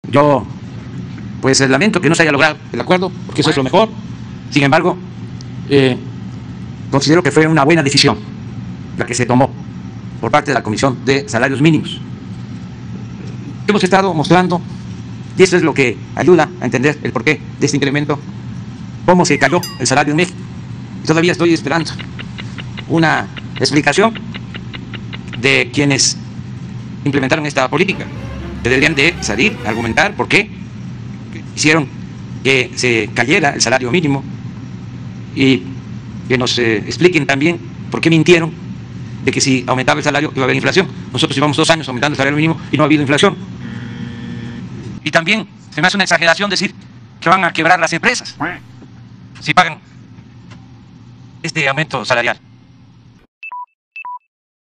Considera que fue una buena decisión aumentar el salario mínimo, niega que provoque inflación 🦆
Considera que fue una buena decisión aumentar el salario mínimo y pide que den una explicación a los que hicieron que se cayera el salario mínimo 🦆” porque no ha habido inflación”